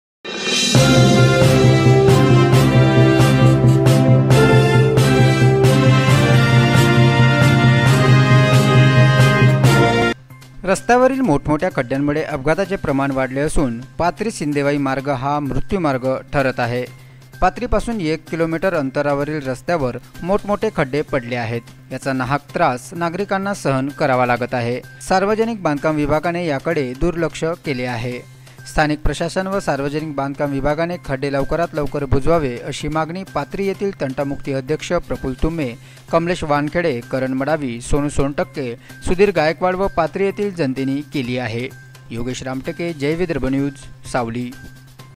पाथरी-सिंदेवाही मार्ग ठरत आहे ‘मृत्यू’मार्ग
रस्त्यावर ठिकठिकाणी खड्डे: खड्डे बुजविण्याची मागणी